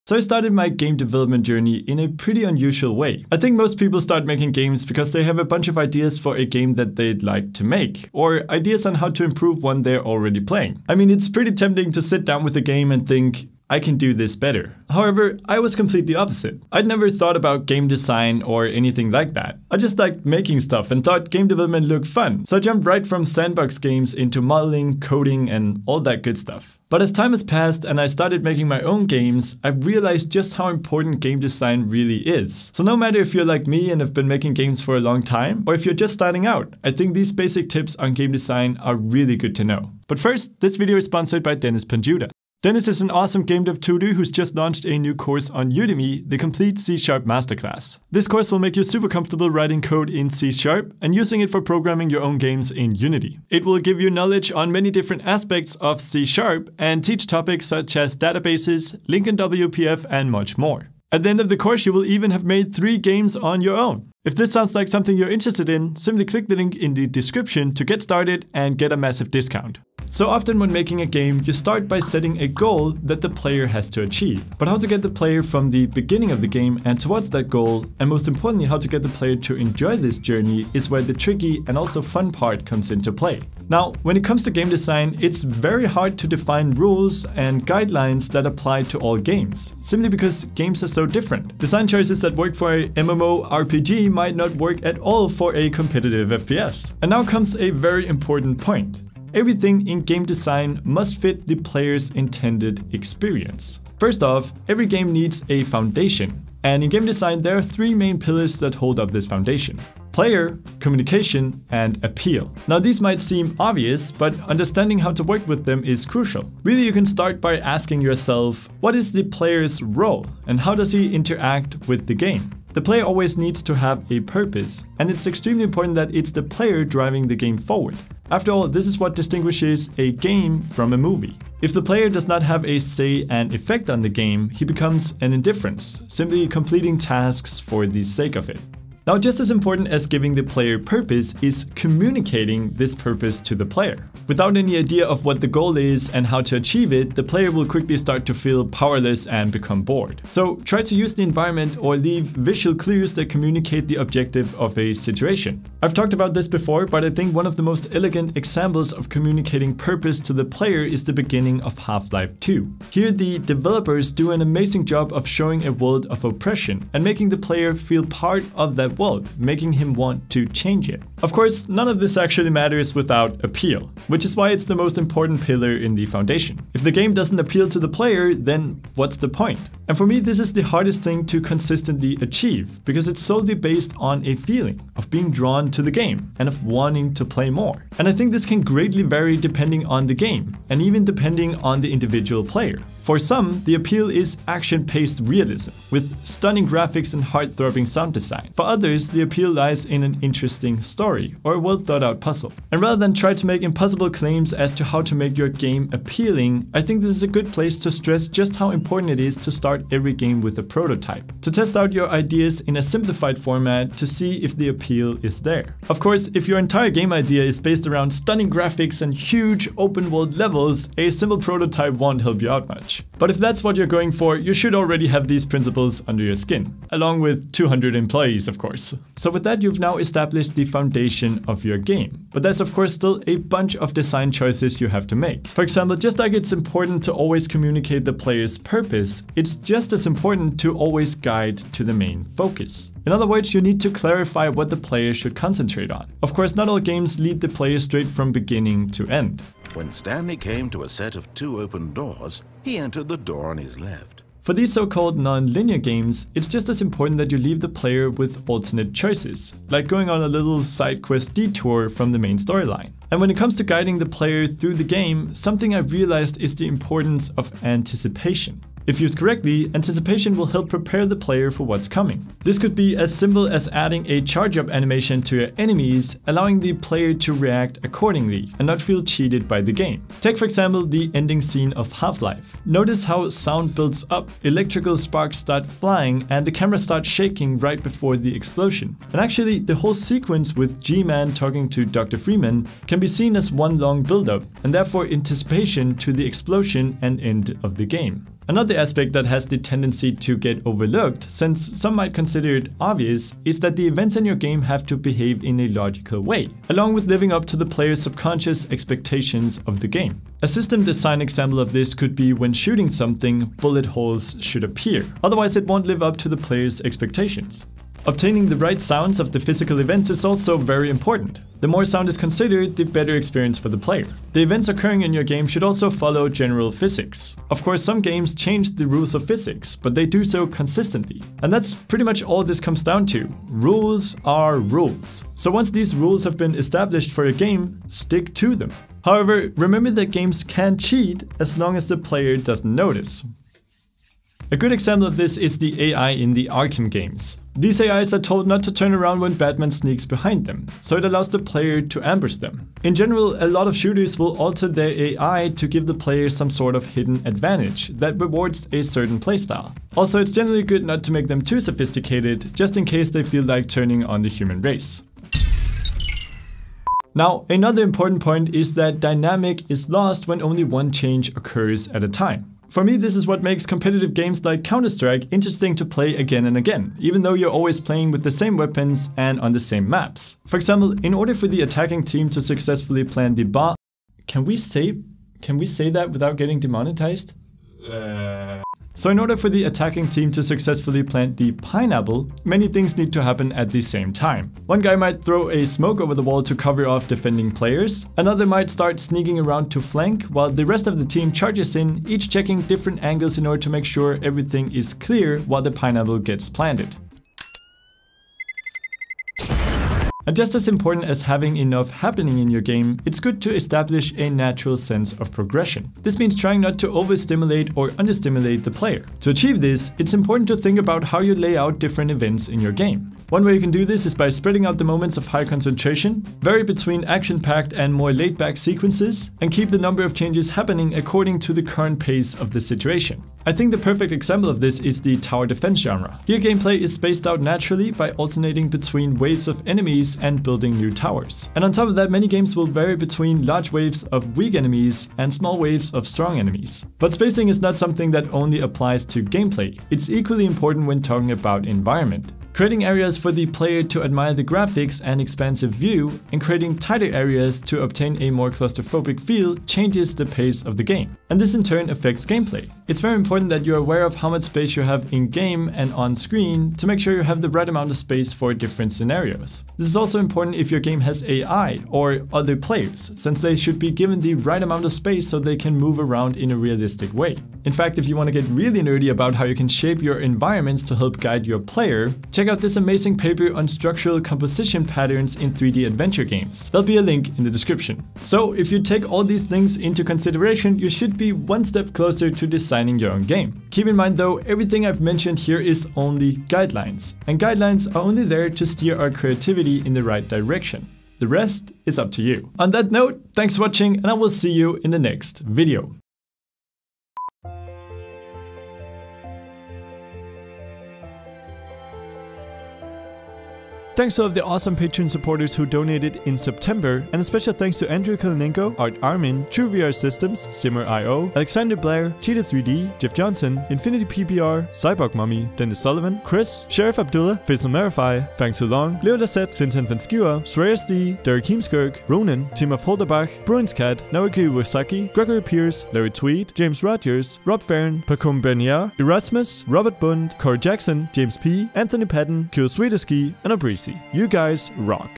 Basic Principles of Game Design
How do you make good games? Huge question, but let's try to break it down!